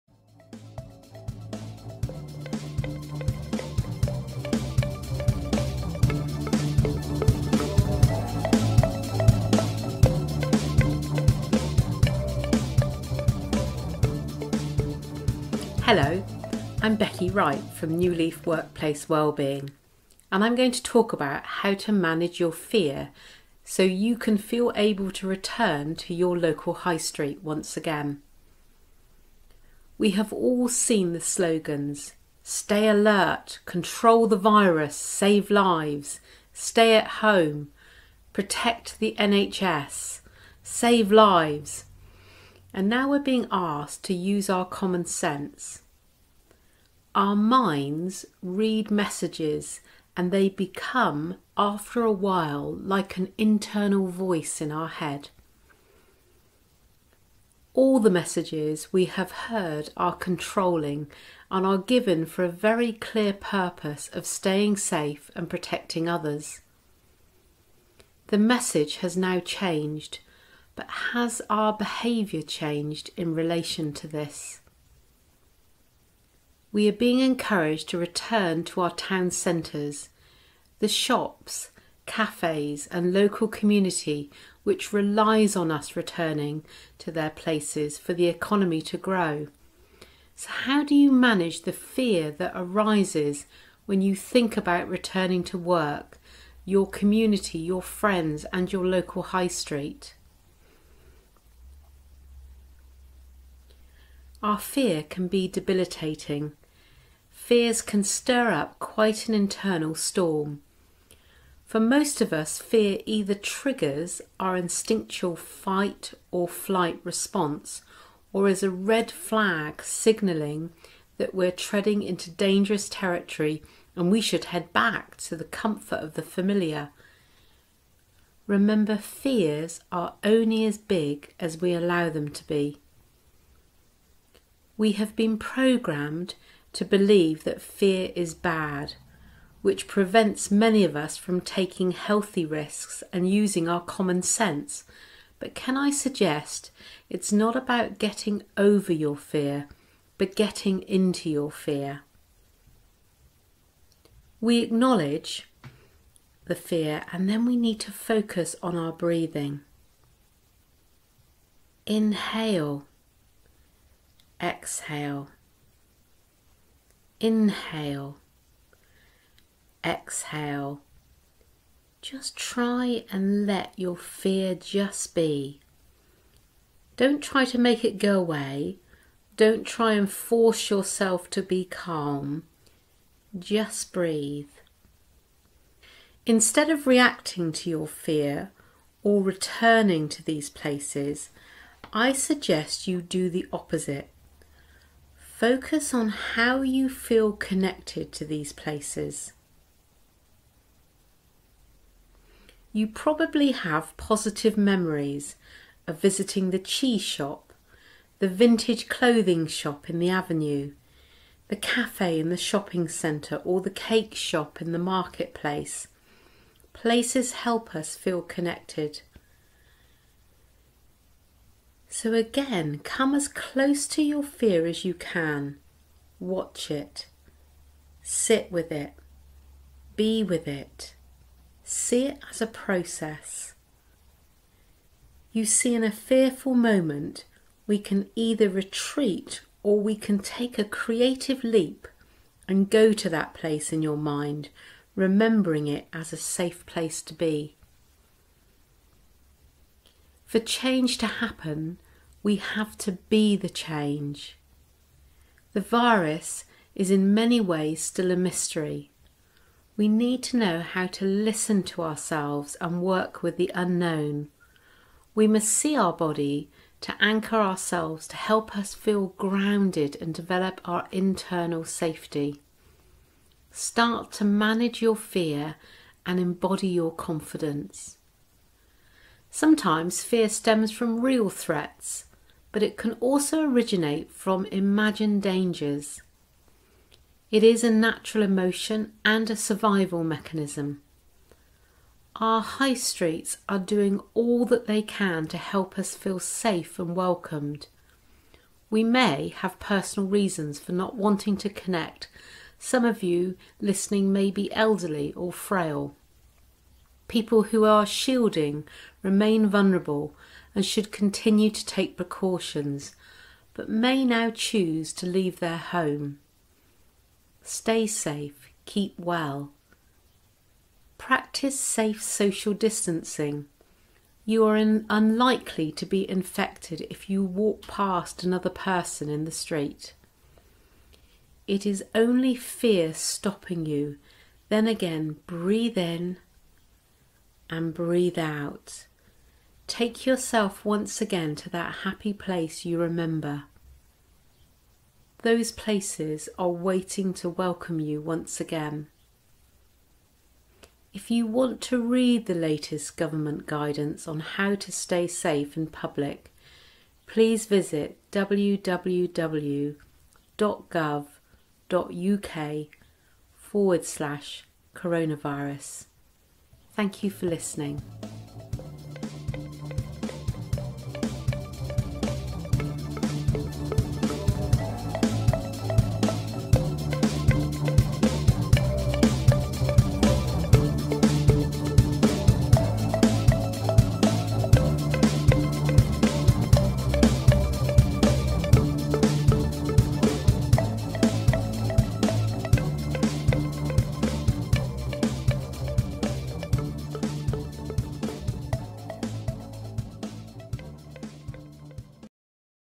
Bridgwater Quayside Festival at Home 2020 Revisit Your High street
Short audio only piece helping people manage anxiety when they return to the highstreet.